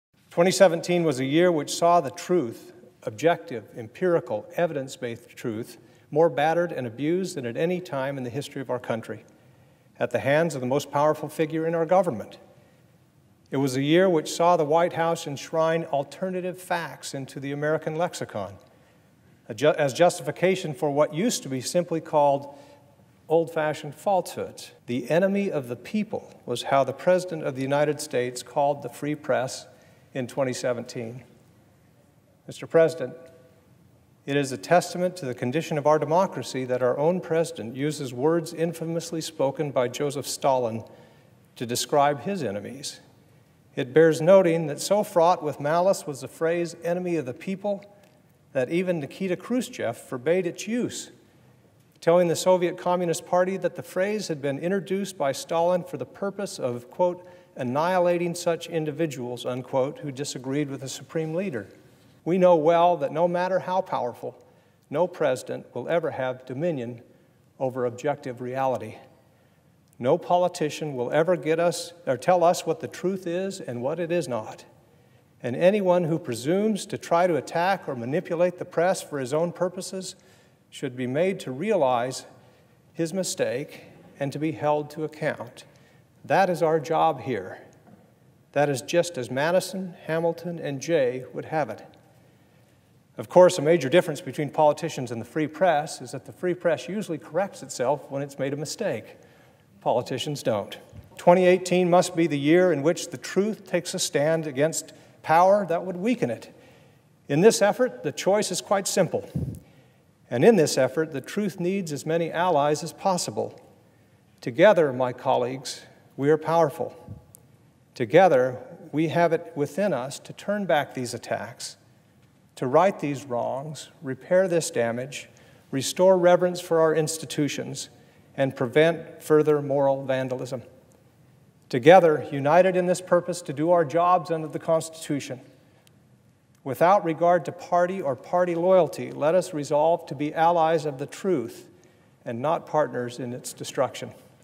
Senator Jeff Flake Likens President Trump's Attacks On Media To Josef Stalin | TIME
Jeff Flake delivered a searing rebuke of President Donald Trump’s attacks on the press on the Senate floor Wednesday, urging the chamber to counteract his rhetoric by defending the media. Flake denounced President Donald Trump's use of the terms "fake news" and "enemy of the people" to describe the news media and stories he doesn't like.